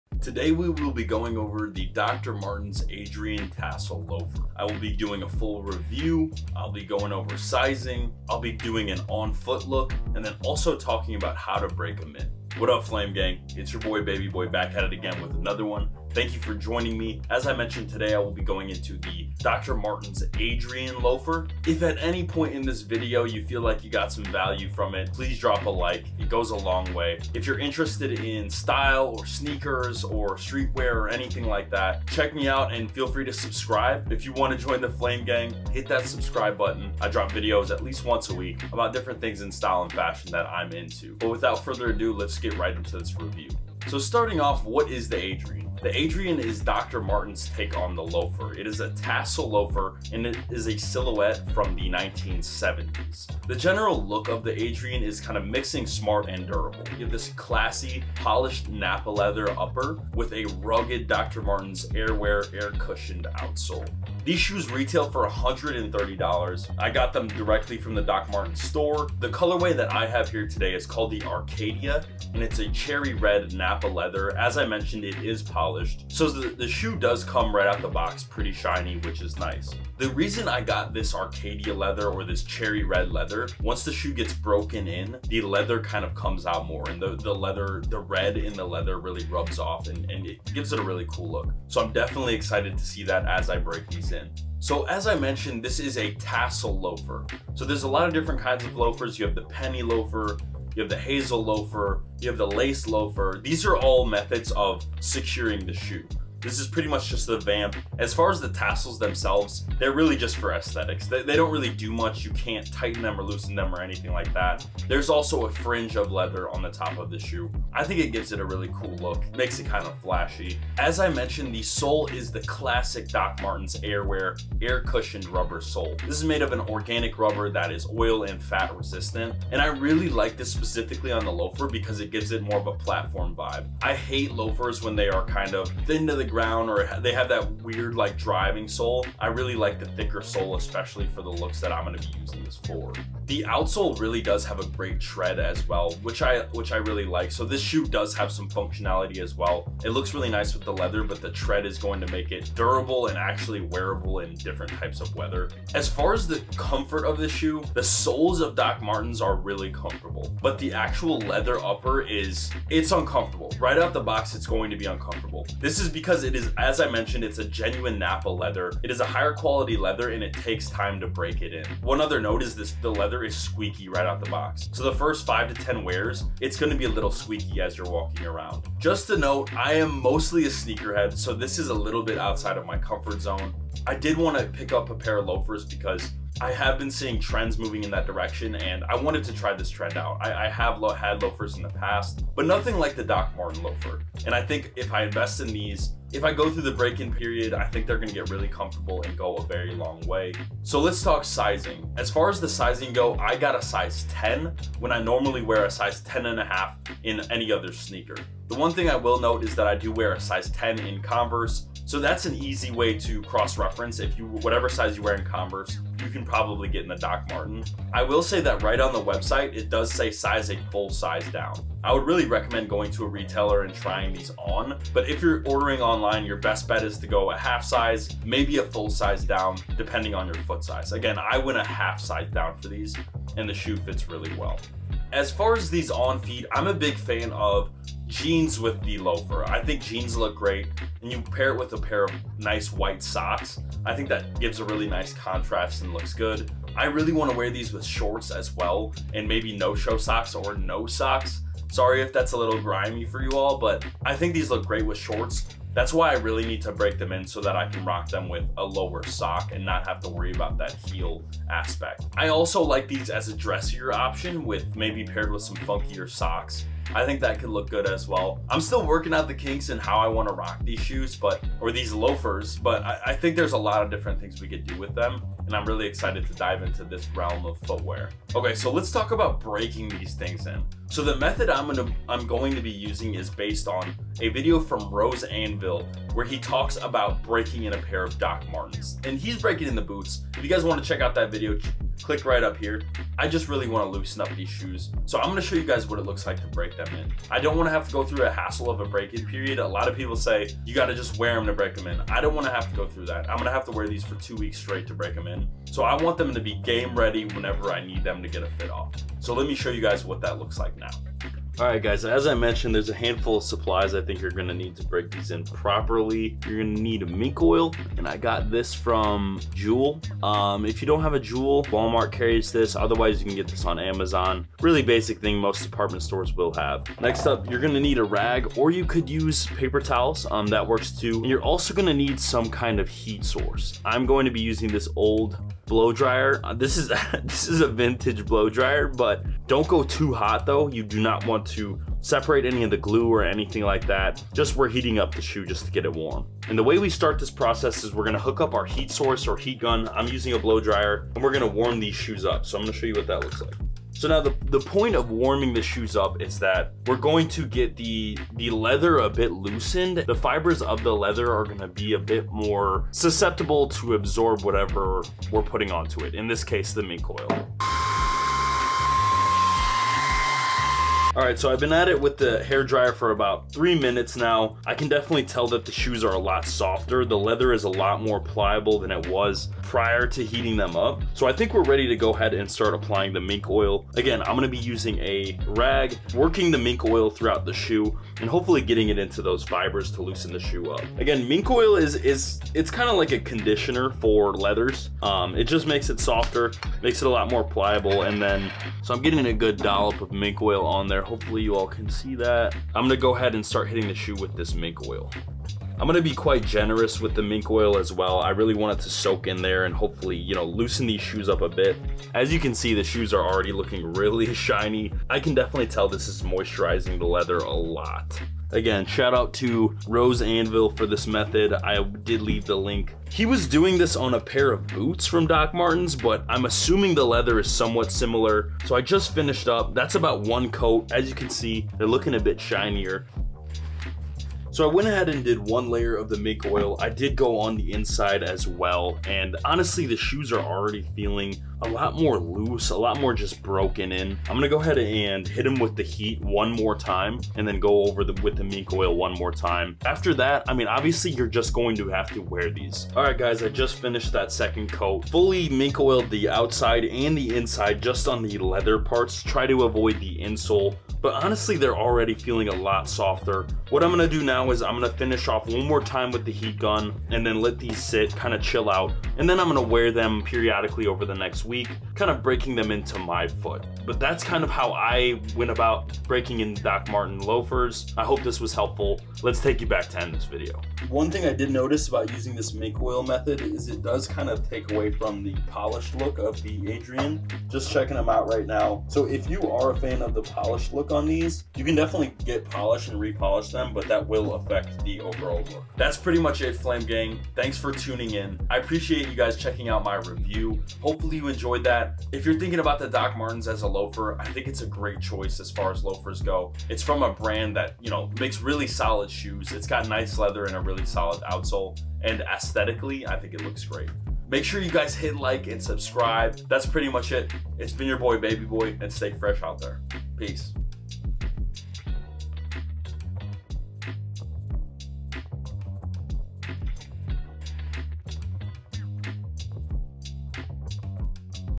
DR. MARTENS ADRIAN TASSEL LOAFER | REVIEW, ON FEET, SIZING, & HOW TO BREAK THEM IN
What's up Flame Gang?? In this video, I will be reviewing the Dr. Martens Adrian Tassel Loafer. I talk a bit about the history of the shoe, show you how the shoe looks on feet, discuss sizing, and provide a method to break the loafers in. If you are thinking about buying this loafer, I would highly recommend it!

00:00 Intro
00:43 Review
03:20 Sizing
03:54 On Feet Look
04:38 How To Break Them In
08:15 Outro